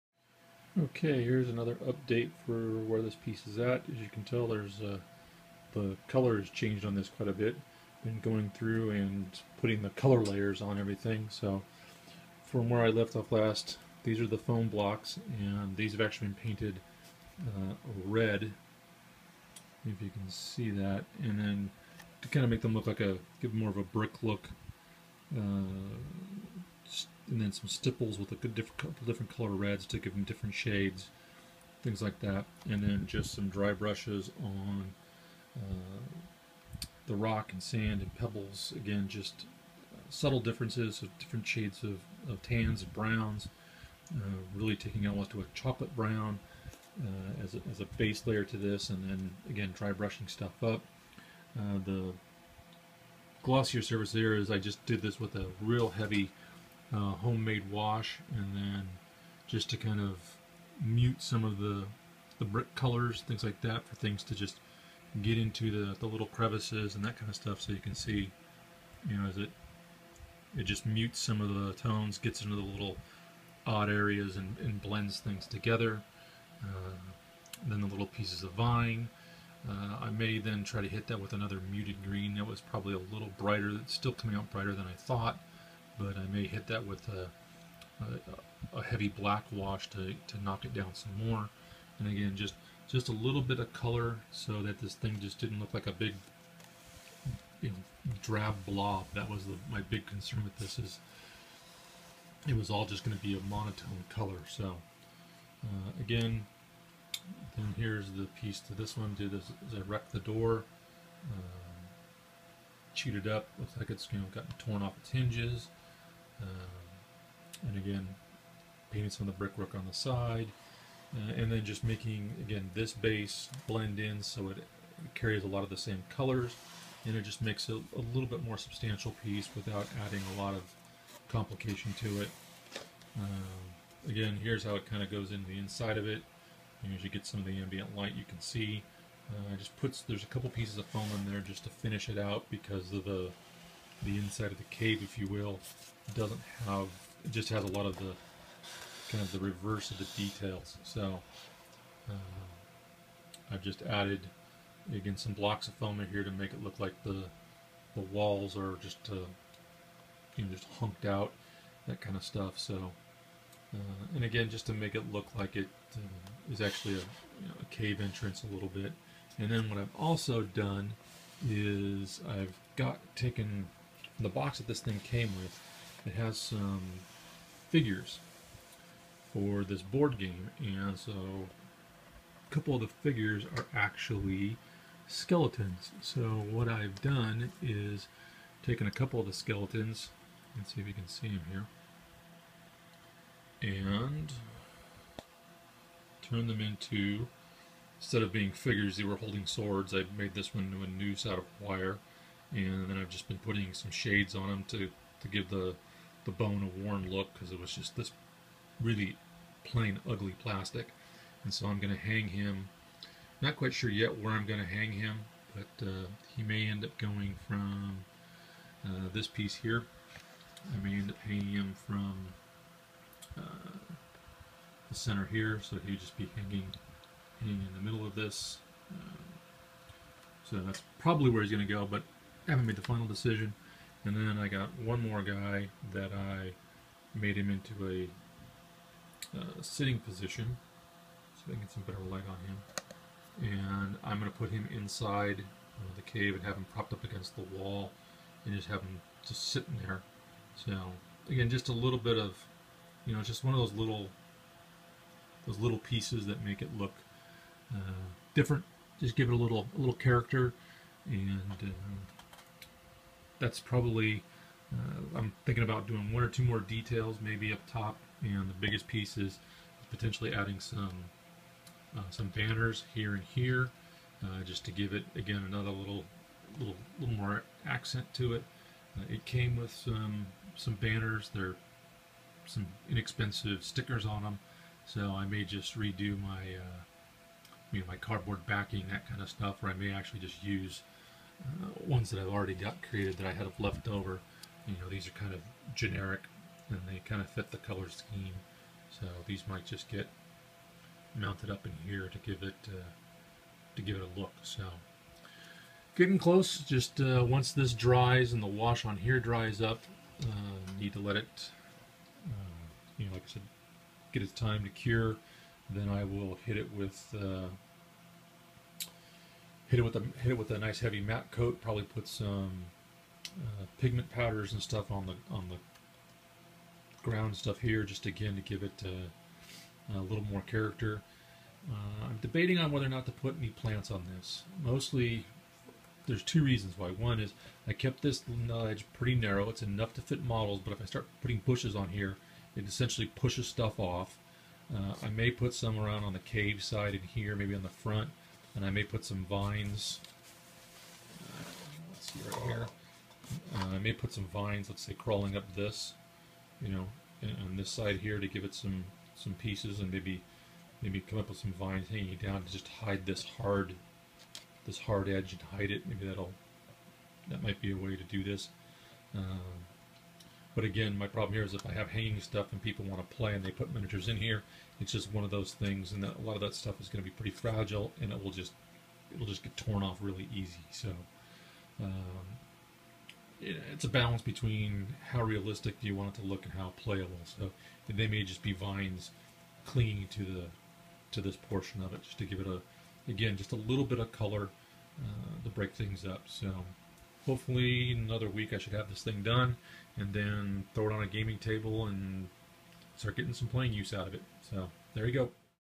Chaos Temple Terrain Build - Part 3
Part 3 - Starting to put colors down while finding a balance.  Hard part is the piece looks monotone -- trying to find the right amount of accent color so it doesn't look like a My Little Pony piece.  Comments & Suggestions are always welcome.